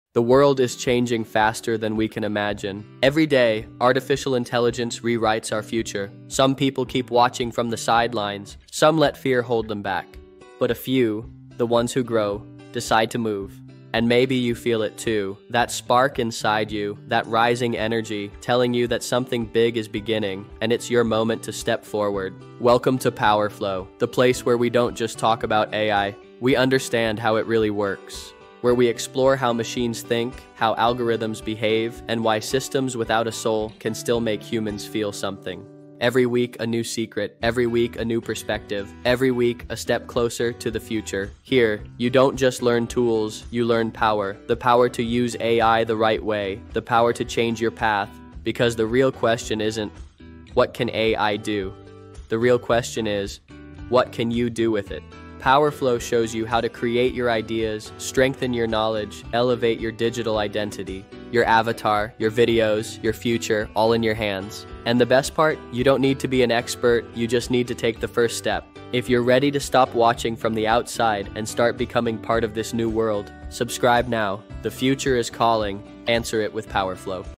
Unlock AI Power
Unlock the power of artificial intelligence — and step into a future shaped by your ideas, your creativity, and your ambition.

In this cinematic Power Flow introduction, you’ll discover:
⚡ how AI is transforming our world
⚡ why now is the moment to grow
⚡ how you can use AI to create, build, and evolve
⚡ the mindset that separates spectators from leaders

This is more than a video.
It’s the beginning of your journey.

The future is calling.
Answer it with Power Flow.